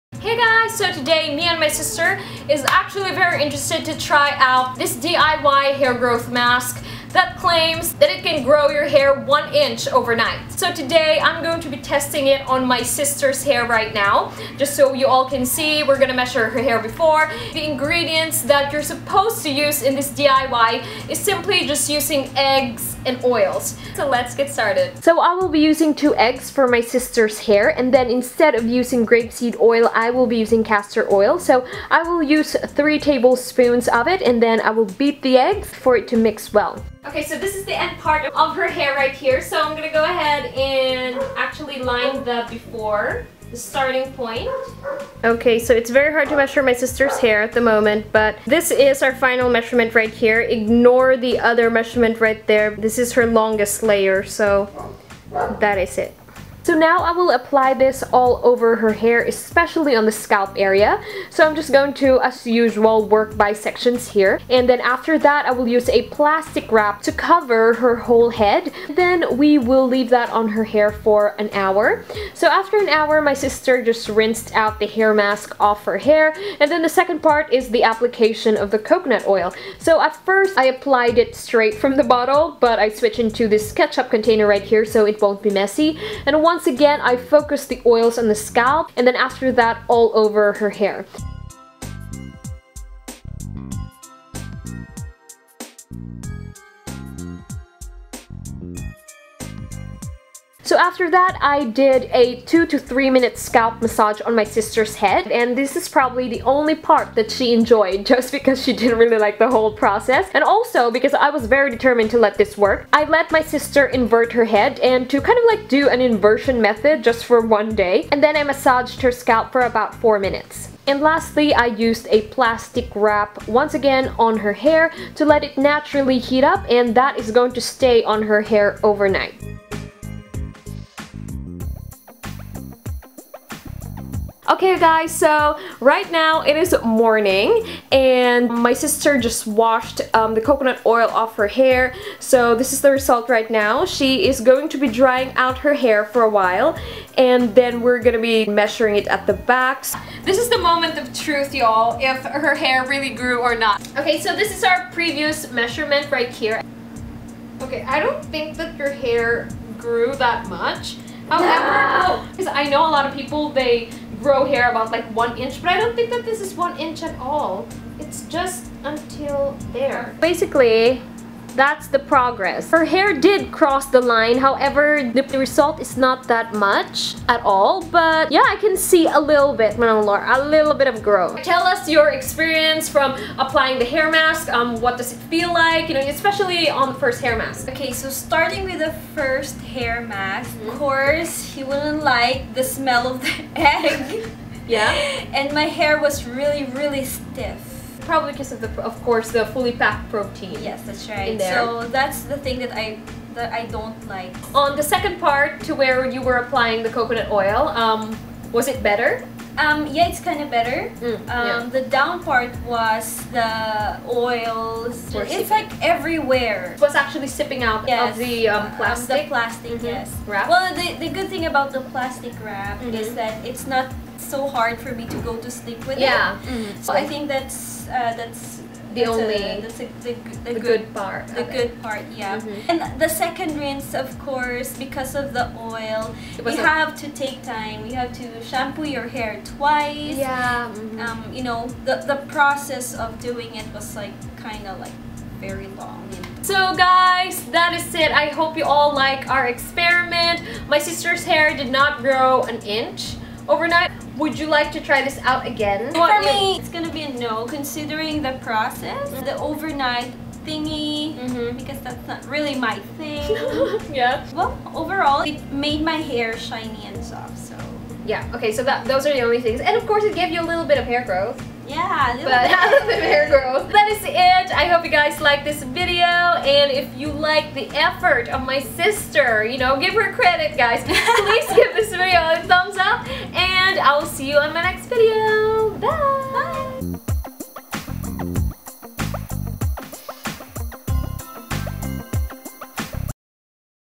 GROWING YOUR HAIR 1 INCH Overnight HAIR MASK?- TESTED-DOES IT WORK? BEAUTYKLOVE
So some of you from last year have been really asking me to try out this hair mask that claims it can grow your hair hair one inch in a day so my sister and I tried it out.